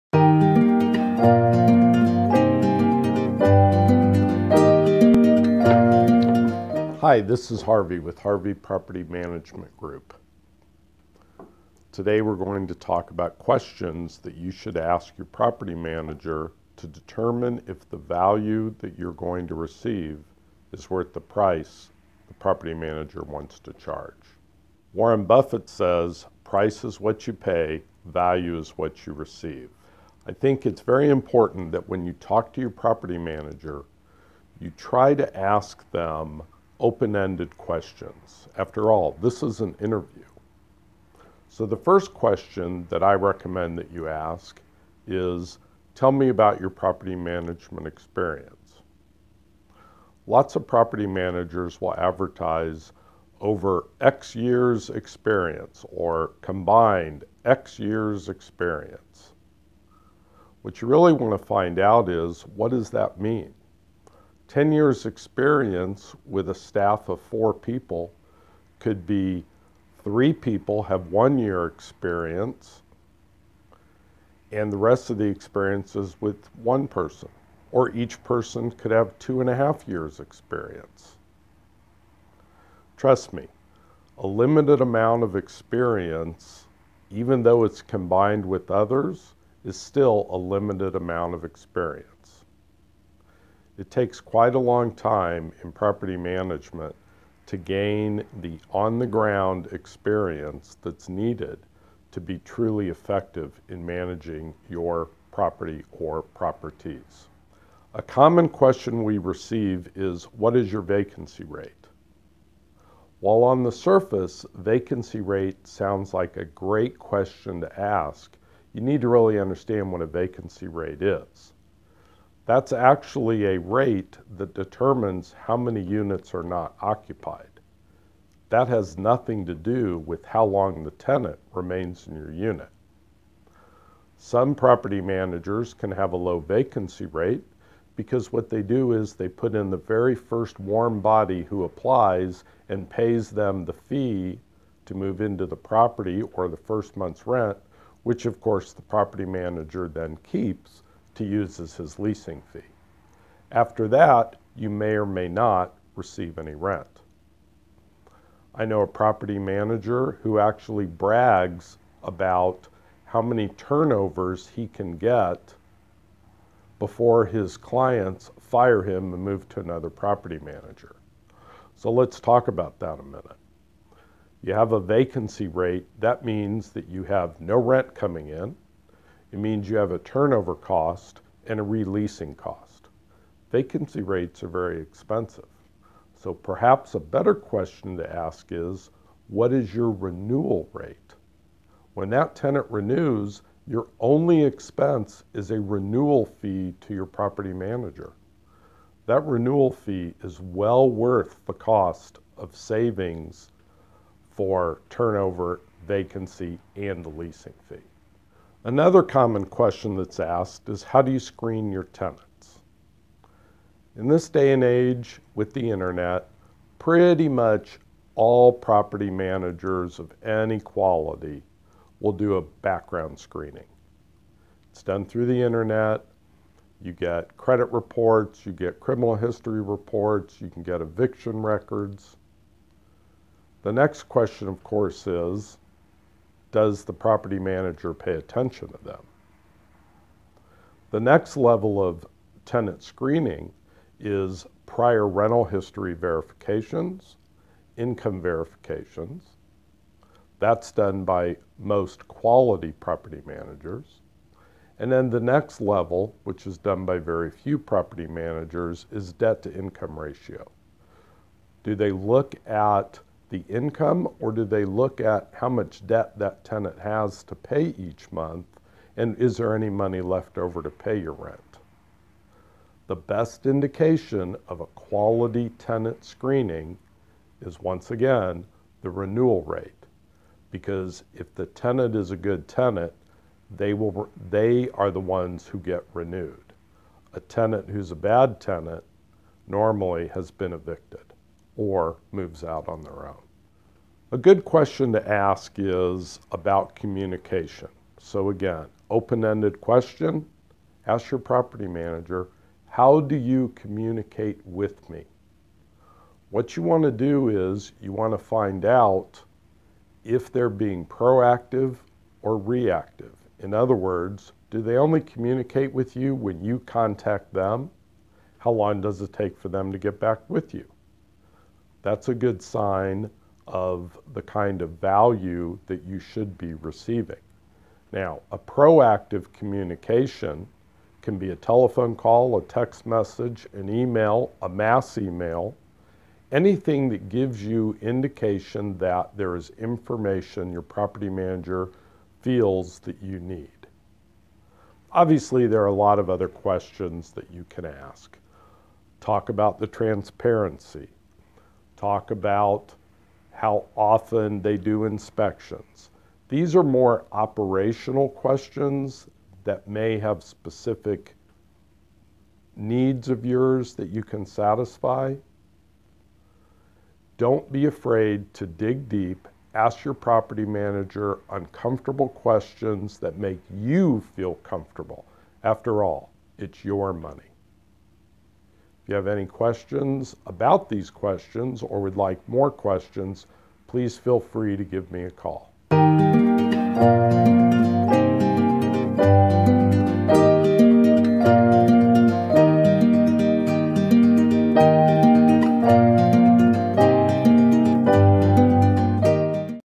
Questions to Ask Rental Property Managers in Indianapolis, IN
There are a few important questions you should ask rental property managers to determine if the value you’re going to receive is worth the price the property manager wants to charge. Warren Buffet says...